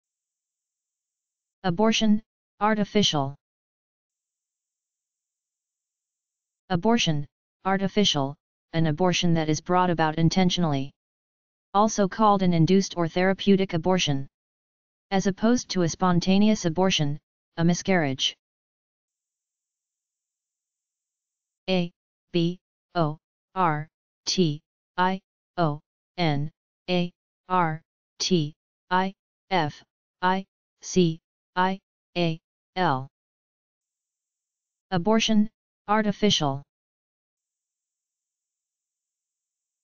Abortion, artificial - Medical Meaning
Abortion, artificial

Abortion, artificial: An abortion that is brought about intentionally. Also called an induced or therapeutic abortion. As opposed to a spontaneous abortion (a miscarriage).